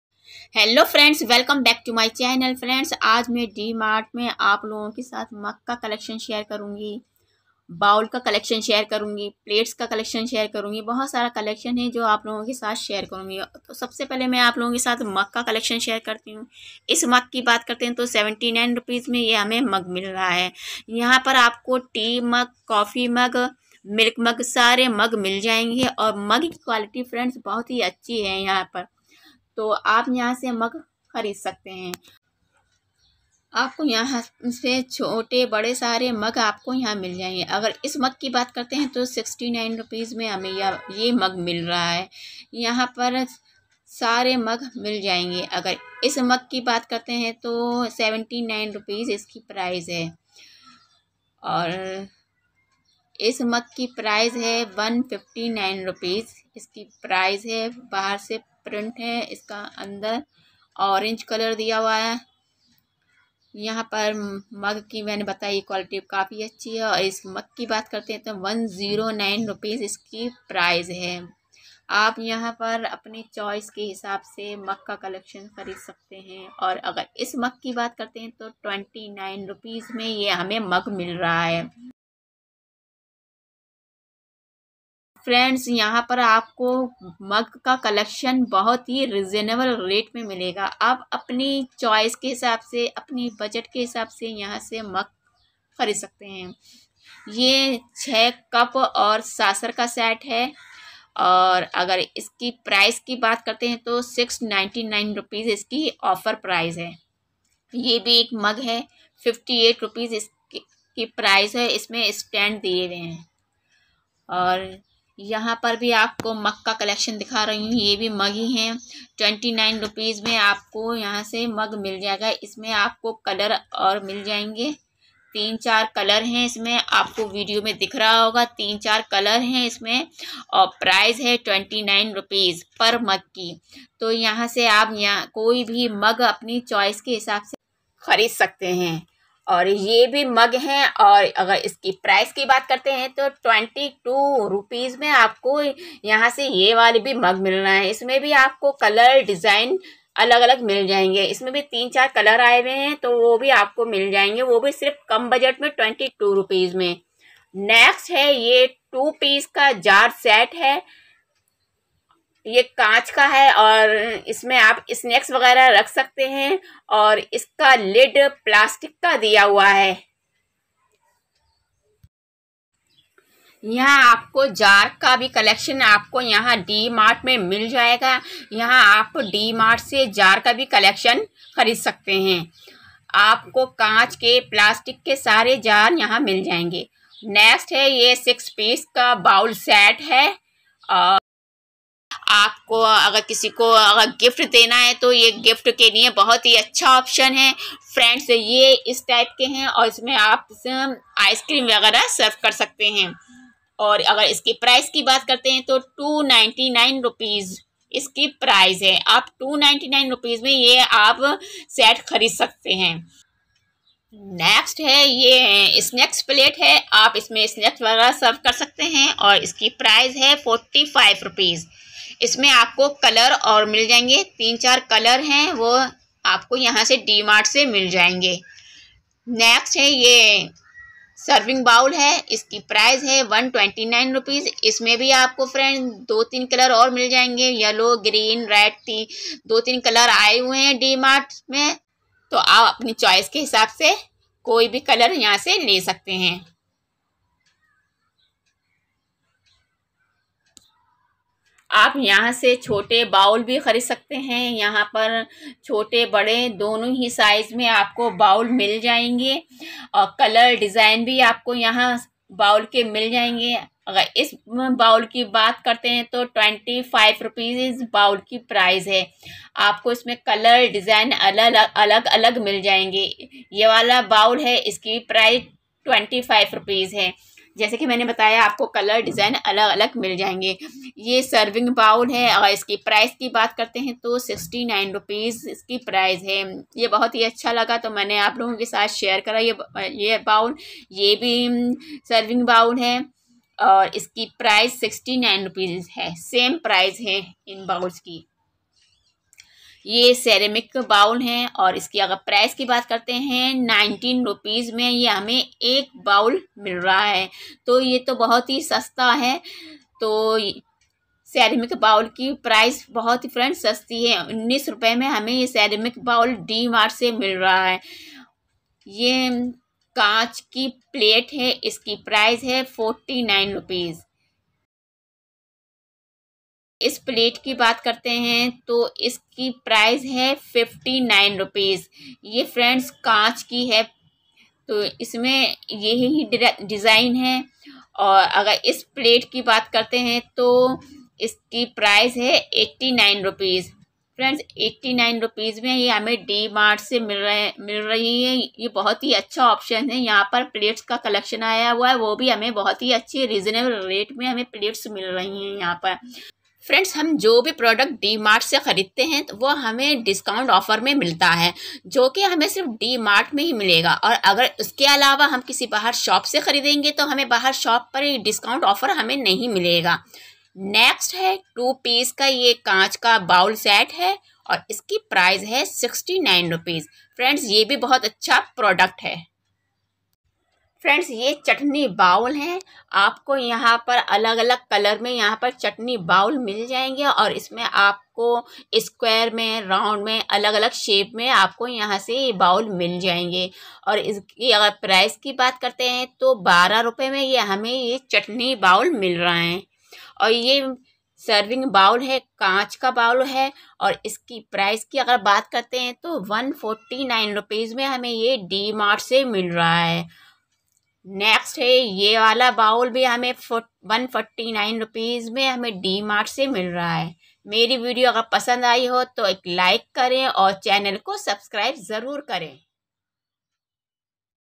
D Mart Veriety starts Rs.12, Bowl , mug, jar, all kitchen products..
D Mart is very big and nice mega store..

This D Mart is located in Navi Mumbai
( Kopar Khairane ).

All home appliances, bathroom accessories, stainless Steel products, kitchen accessories, stationeries, readymade garments, footwears, and totally house hold items are available under one roof " D Mart ".

There are daily saving offers in DMart.

Cheapest store D Mart.